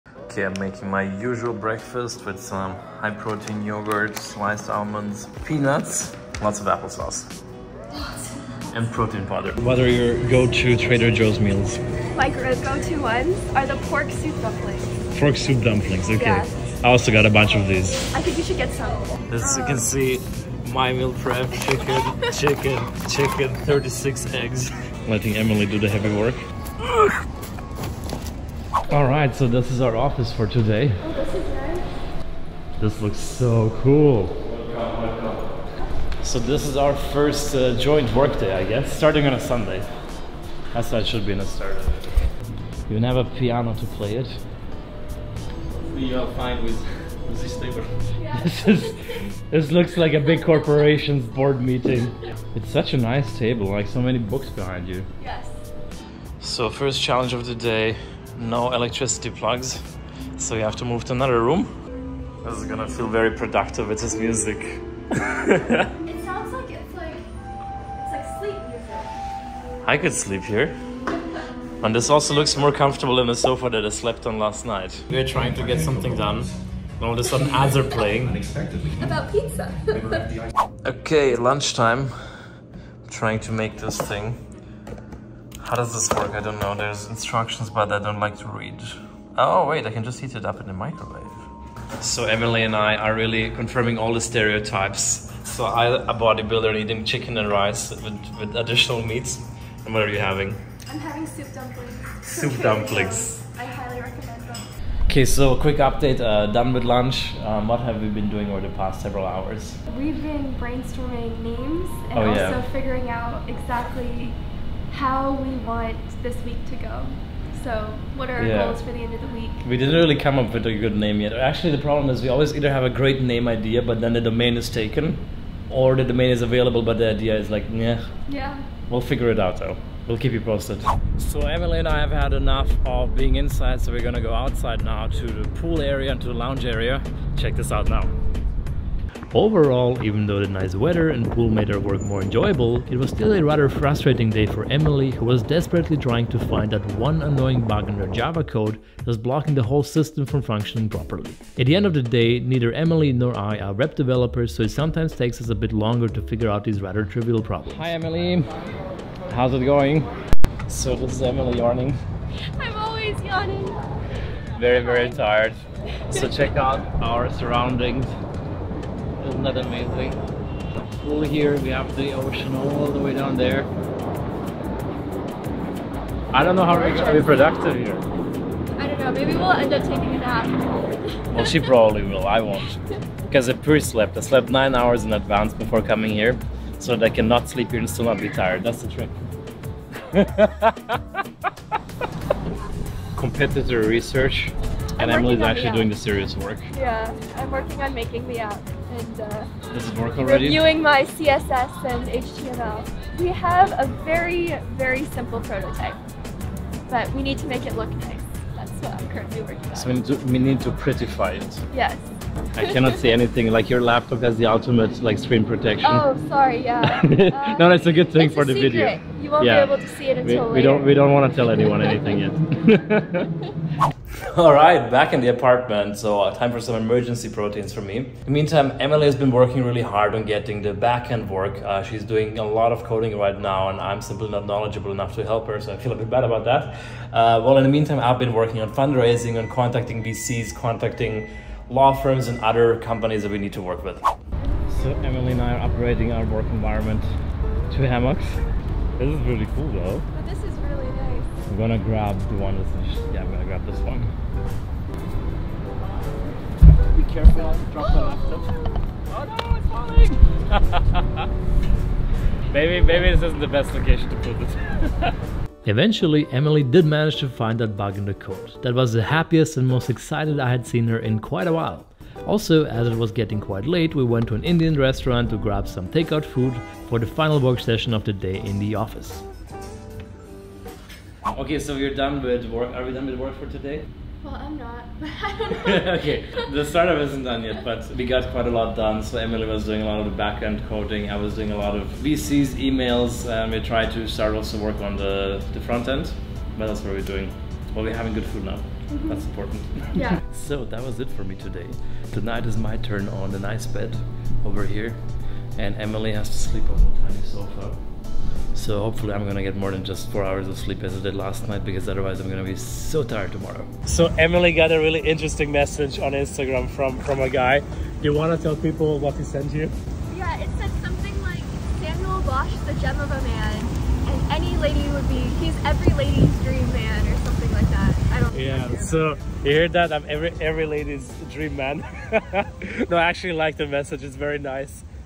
Pool Time and Debugging Code (Ep. 3)
Day in the Life of an MIT Startup Founder. This is episode #3

This is the first day of our startup trip to Miami. My friend and co-founder Emily Mu and I are staying at our third co-founder's apartment for a week to develop a working prototype of our startup. Our startup is a legal tech company making lawyers more productive using large language models (LLMs), such as GPT-4 and ChatGPT. We haven't raised any money yet and are actively talking to VCs and law firms.

▬ Timestamps ▬▬▬▬▬▬▬▬▬▬▬▬▬▬▬▬▬▬▬▬▬▬▬▬
0:00 - Breakfast and grocery shopping
0:29 - Getting started with work
1:20 - Quick lunch
1:44 - Brainstorming a startup name
2:11 - Working outdoors
4:03 - Debugging code
4:32 - Dinner and last work session
5:13 - Outro